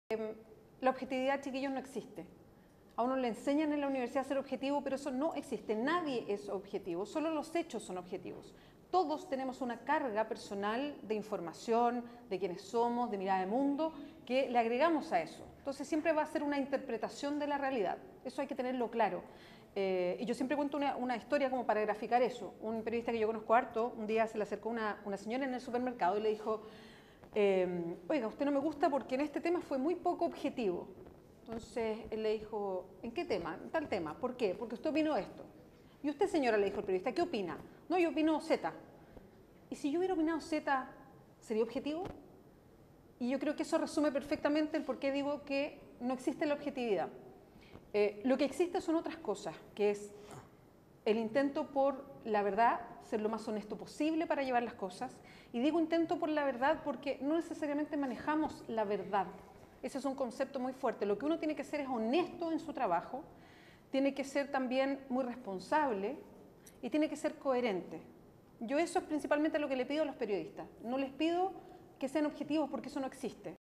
Beatriz Sánchez: "La objetividad no existe"
Extracto de la charla "Desafíos de la radio en la actualidad y periodismo de opinión", dictada por la periodista Beatriz Sánchez en la Universidad Finis Terrae.